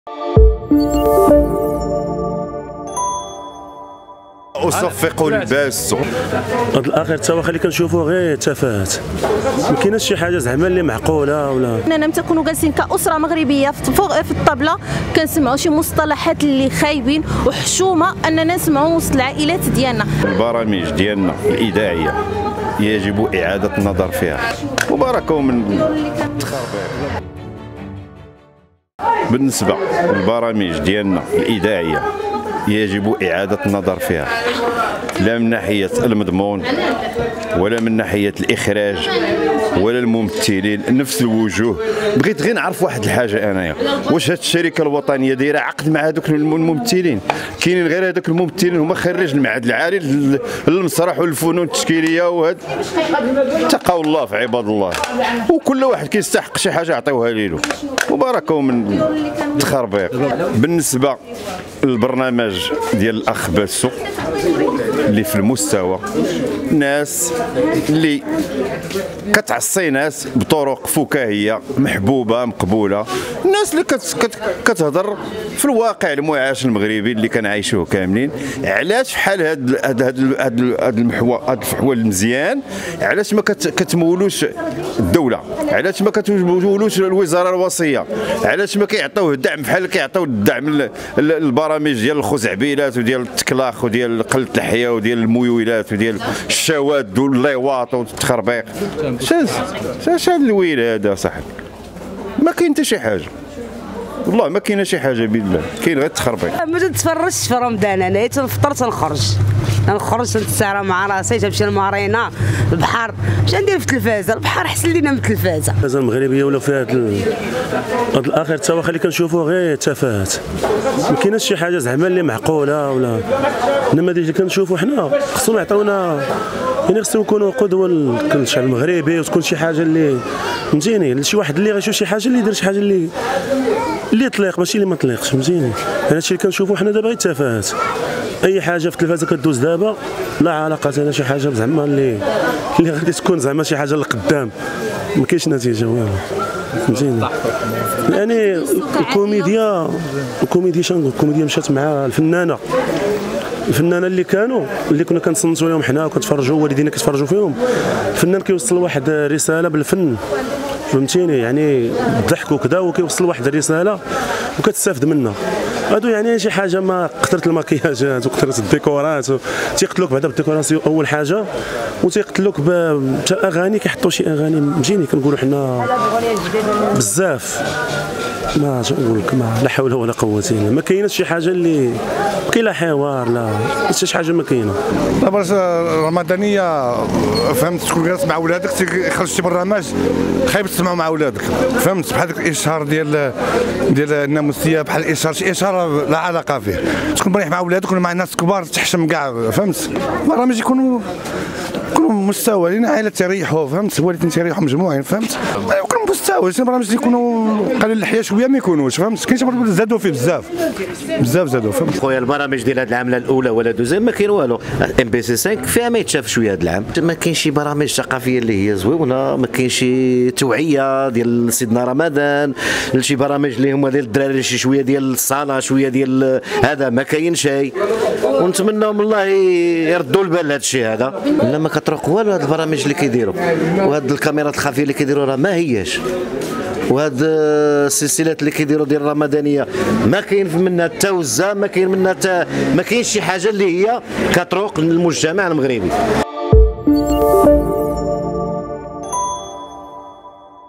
مواطنون يستنكرون " تفاهة" البرامج الرمضانية
مع حلول شهر رمضان، تعلن القنوات عن برامج خاصة بهذا الشهر..لكن يصدم المشاهدون بأن معظم تلك البرامج تافهة..تعرف على رأي مواطنين استقت " كاميرا أنفاس بريس" تصريحاتهم.